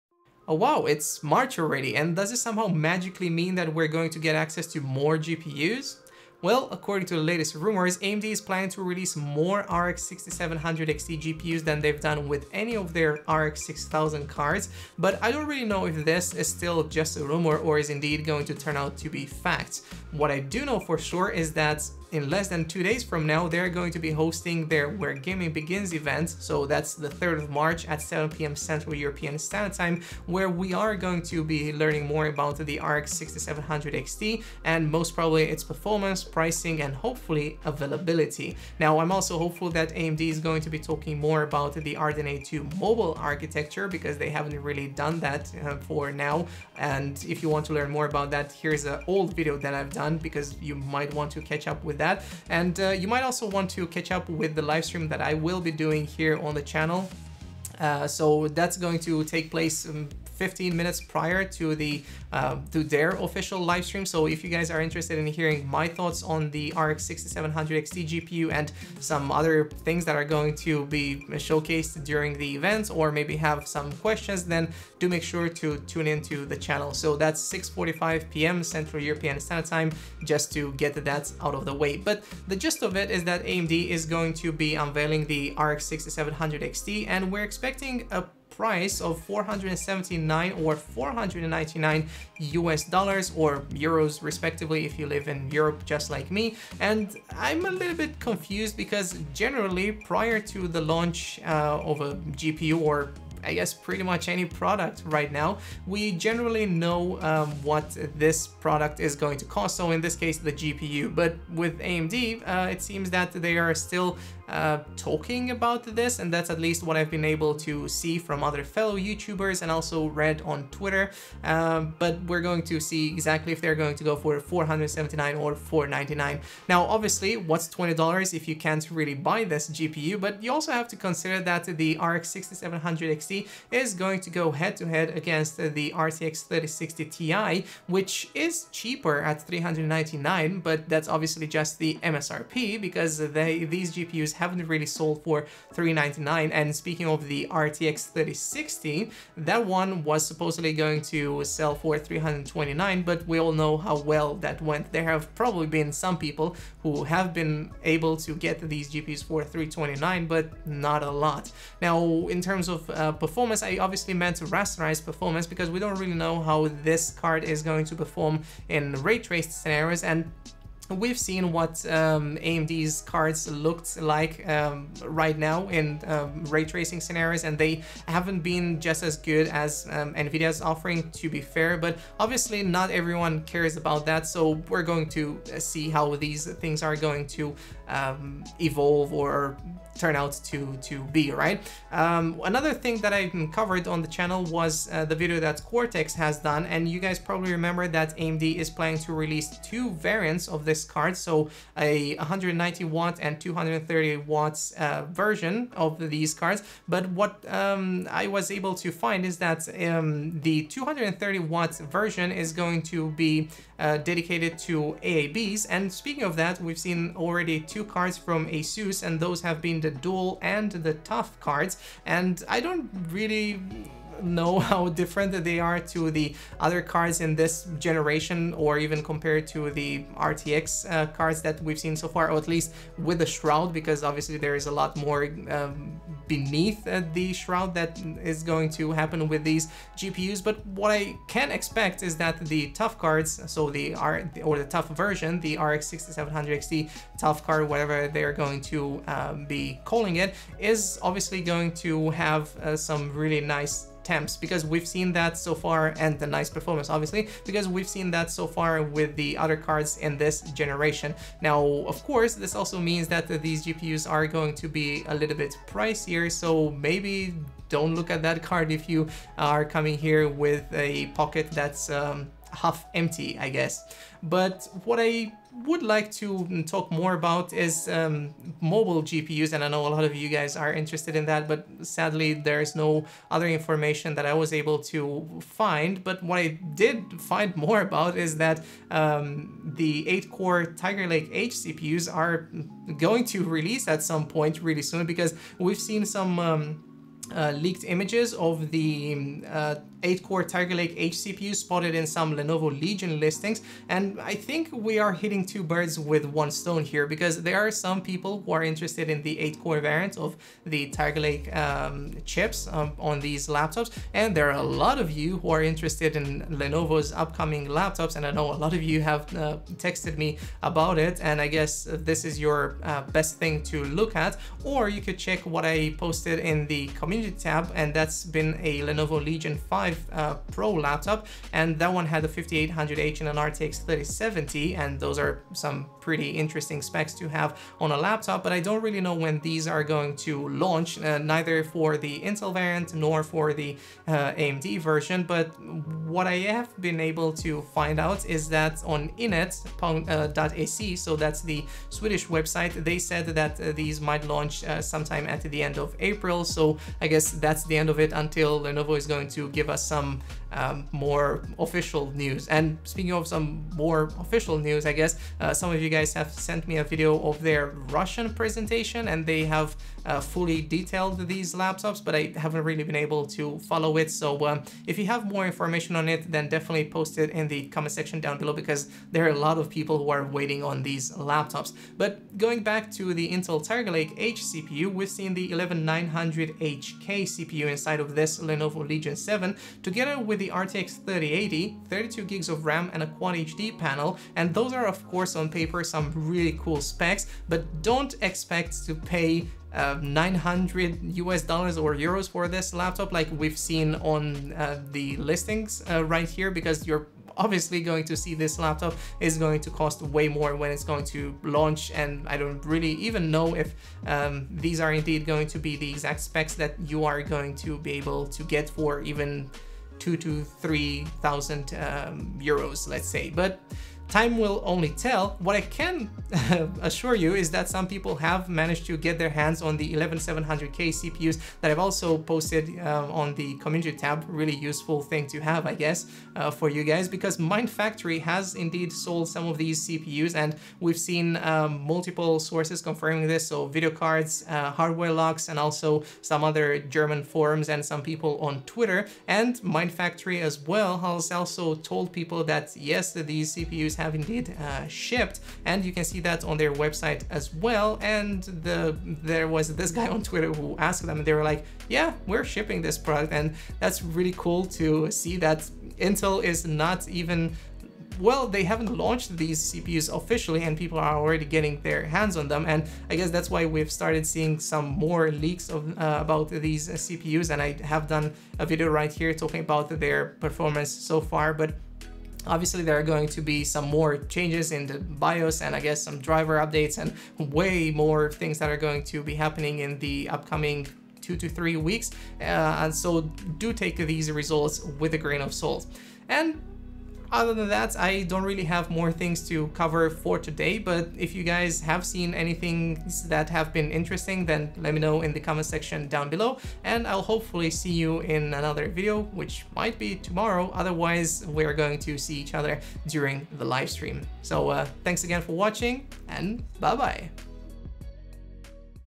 RX 6700XT pricing & Lenovo Legion laptops with 8 Core Tiger Lake CPUs spotted
AMD will unveil the RX 6700XT GPUs at their Where Gaming Begins - Part 3 event and there have been more rumors regarding pricing and availability which I'd like to cover in this video. Will this finally be a GPU you can buy?

If not, hopefully you will soon be able to buy the Lenovo Legion laptops, featuring the 8-core Tiger Lake-H CPUs or one of their RYZEN variants (with a RYZEN 5800H and RTX 3070 mobile), because we've seen those in the wild as well. Believe it or not, some people have been able to pre-order them way before launch! But it's not like we haven't seen this already with Intel's 11700K (with over 100 units sold on Mindfactory.de alone).

✅ Amazon DE
✅ Amazon US
✅ Amazon UK

Timestamps:
0:00 - AMD RX 6700XT launch, specs, pricing and more
4:48 - 8-Core Intel Tiger Lake-H and upcoming Lenovo Legion laptops
7:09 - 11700K already shipping to customers
8:17 - Closing thoughts